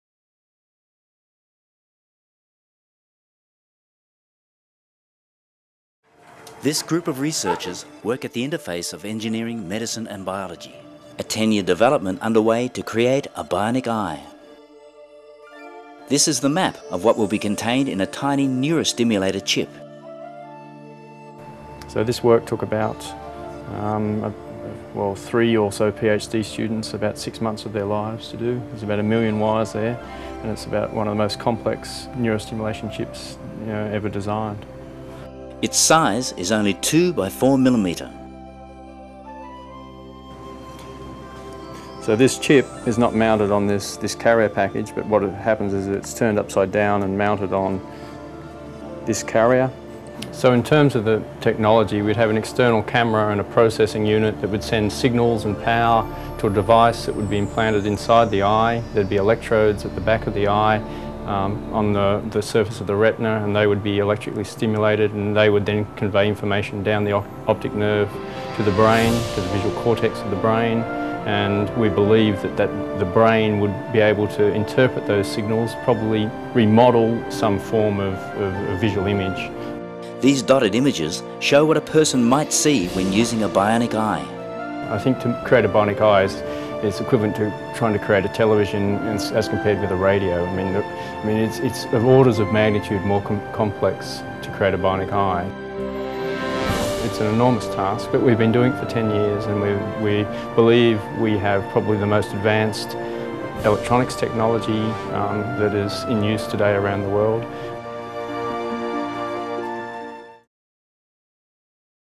Bionic Eye by 2020
A working bionic eye could be an Australian world first by 2020 if action is taken quickly, leading researchers at the University of New South Wales say. Featuring  Professor Nigel Lovell from UNSW's Graduate School of Biomedical Engineering.

Professor Nigel Lovell
UNSW Graduate School of Biomedical Engineering
Australian Vision Prosthesis Group